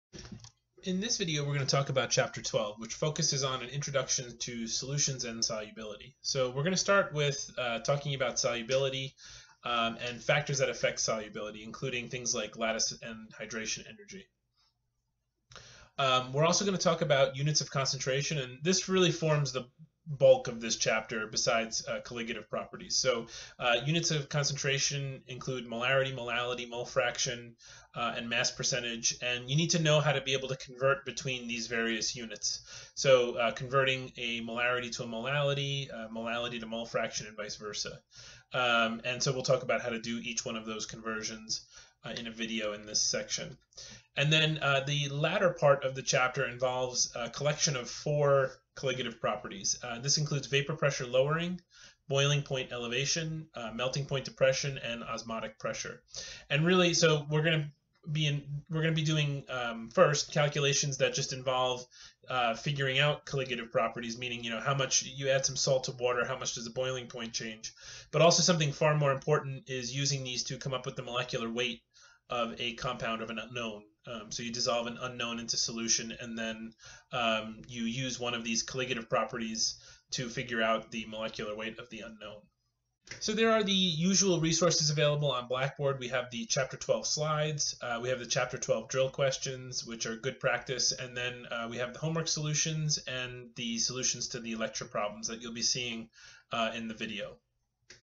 Chapter 12. Introduction
Chapter 12 provides a basic introduction to solutions, units of concentration, and colligative properties.